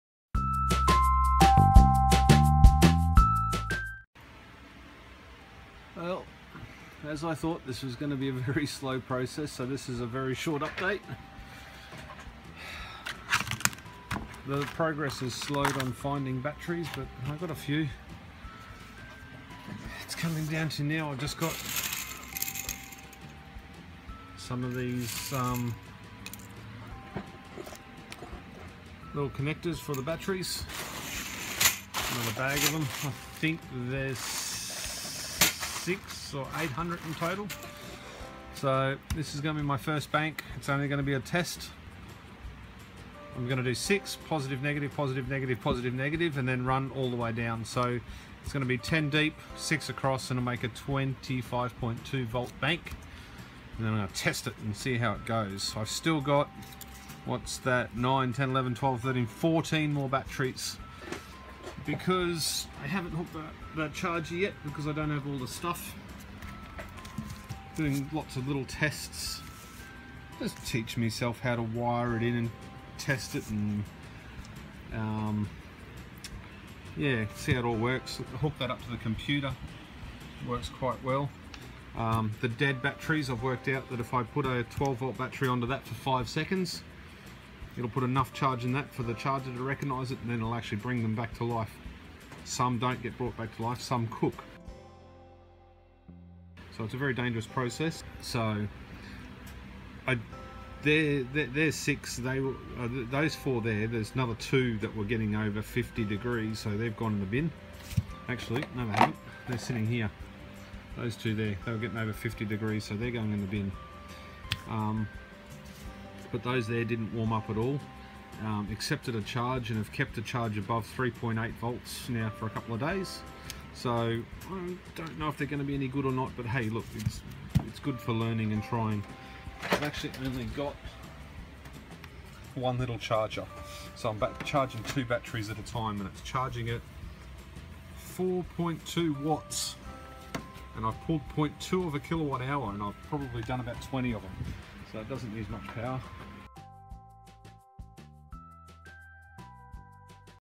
Homebrew Powerwall Project - S01E04 Starting to get rolling with this 18650 project
If anyone has some old 18650 batteries laying around I'd be happy to take them off your hands. It would really help this project proceed much faster.